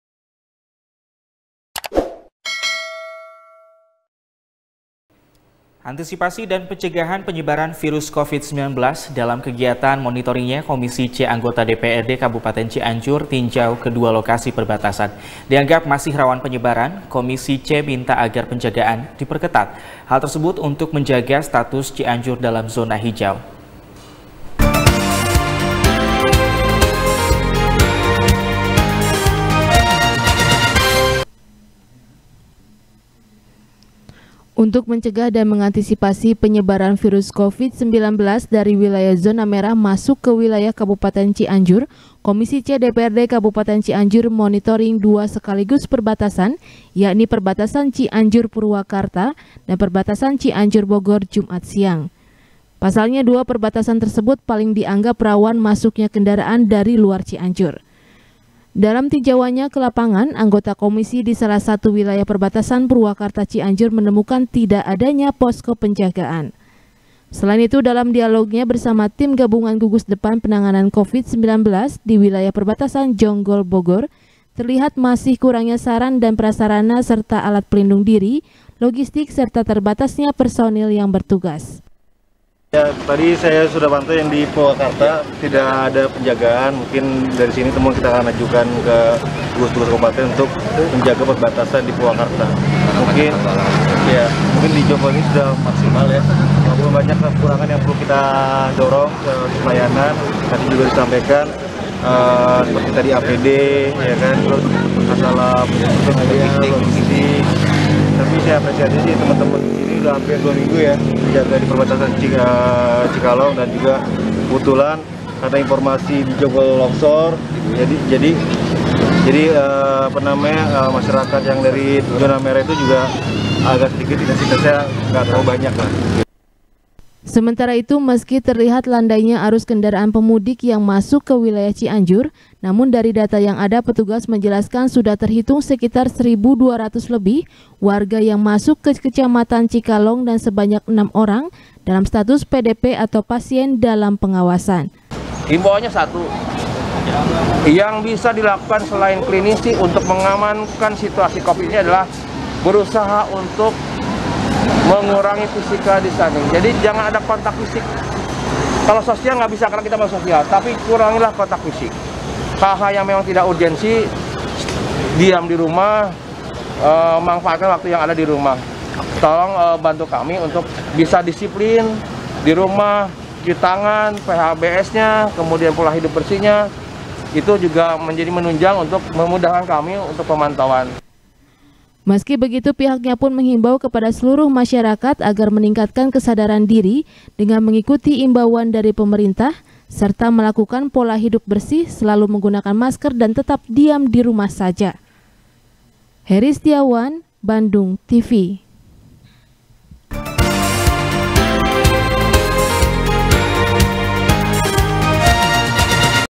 Antisipasi Covid 19, Komisi C Monitor Perbatasan | SBR | BANDUNGTV
SBR | BANDUNGTV | IRFAN RAMANDA | Antisipasi Dan Pencegahan Penyebaran Virus Covid 19 , Dalam Kegiatan Monitoringnya Komisi C Anggota Dprd Kabupaten Cianjur Tinjau Ke Dua Lokasi Perbatasan . Dianggap Masih Rawan, Penyebaran Komisi C Minta Agar Penjagaan Diperketat ,Hal Tersebut Untuk Menjaga Status Cianjur Dalam Zona Hijau .